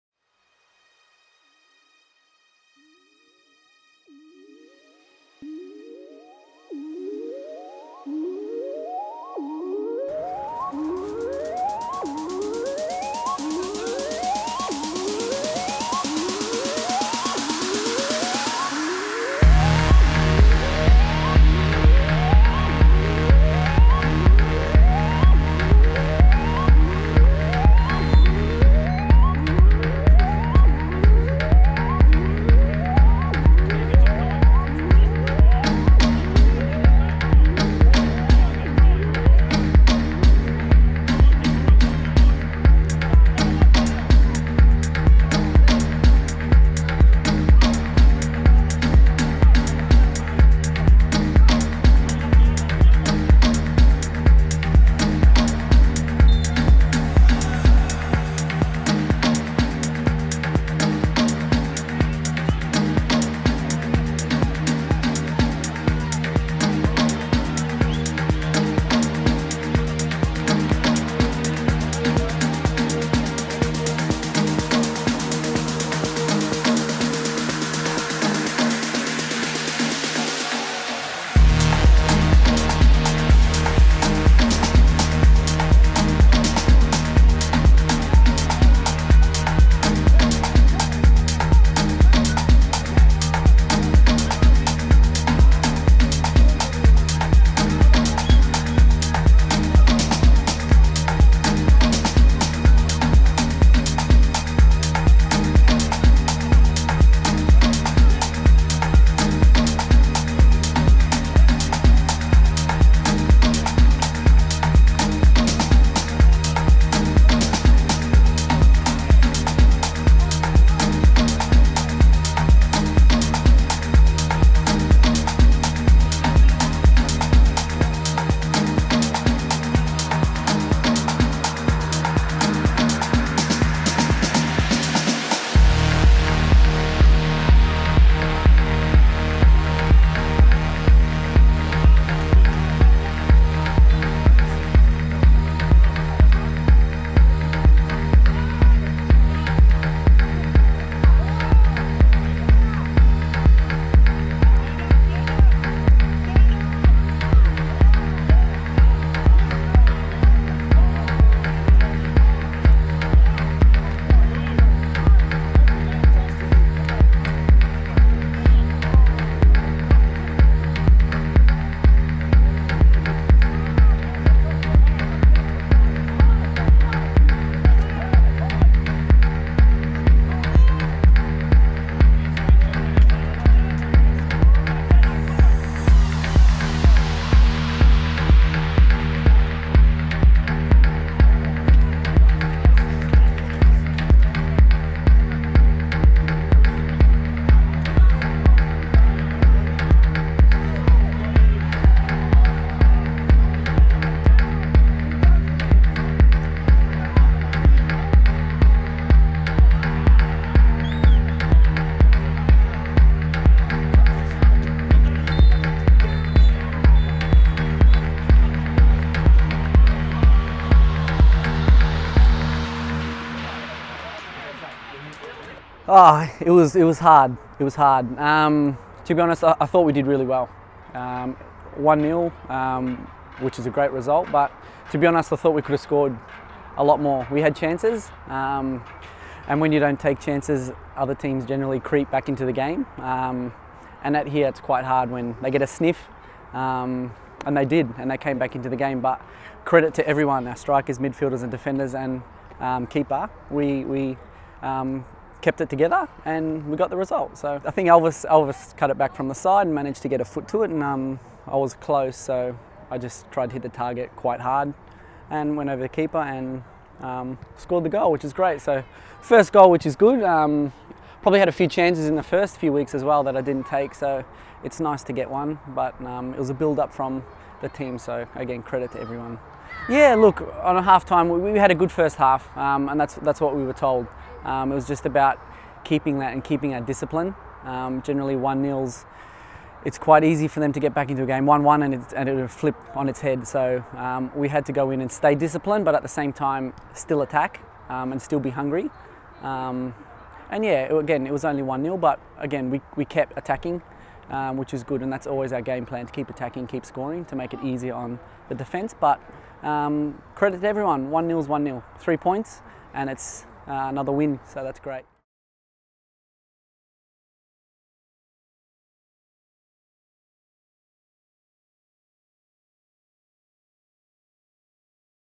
2018 NPL Victoria (Round 6) - Dandenong Thunder vs Avondale - Highlights | 07.04.2018
Catch all the action and reaction from our 1-0 win over Dandenong Thunder in NPL Victoria.

Goal Scorer: E. Christodoulou (36')